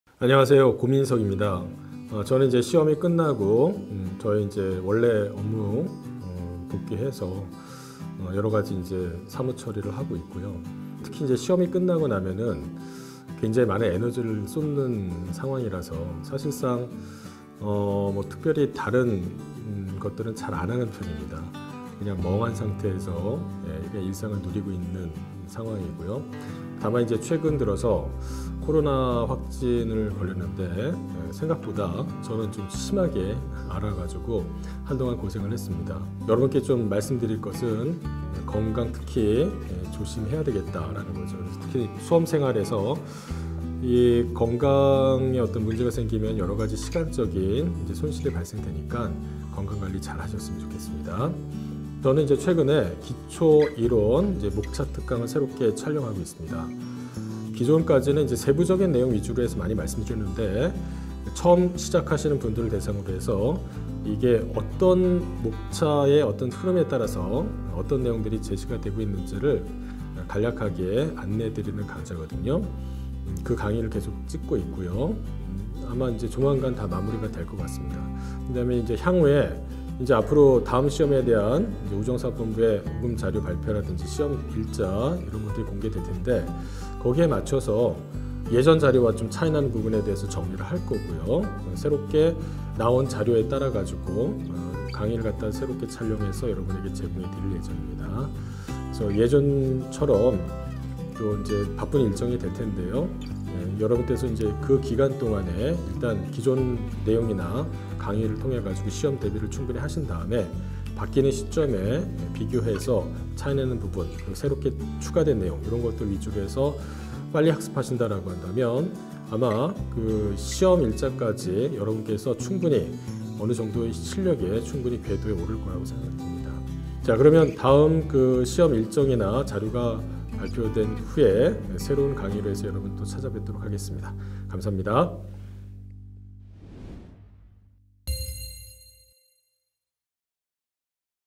[근황소개] 안녕하세요. 고민석 입니다.
[유상통] 우편/예금/보험일반을 맡고 있는 고민석입니다.

저는 최근 코로나 확진으로 인해 약 한달 가량 애를 먹었습니다.
주변분들은 감기처럼 지나가서 큰 걱정을 하지 않고 있다가,
막상 확진 받으니 생각보다 휠씬 아프고 힘들었습니다.

고열과 오한, 콧물 그리고 기침까지
독감처럼 1~2주 고생했고 지금도 기침이 조금 남아 있는 상황입니다.

사람마다 질병에 대한 반응이 다르니 여러분께서도 방심하지 말고
건강에 유의하면서 공부하시길 바랍니다.

저는 요즘 기초이론-목차특강을 촬영 중에 있습니다.
전체적인 내용을 빠르고 가볍게 살펴보는 강의입니다.
아래 이벤트와 선생님 근황 보러가기! 에서 확인해주세요!

선생님의 쏘는 릴레이 선물 이벤트 바로가기

선생님의 근황 보러가기!

계리직 6년 연속 합격자 수 1위 유상통! 바로 가기!!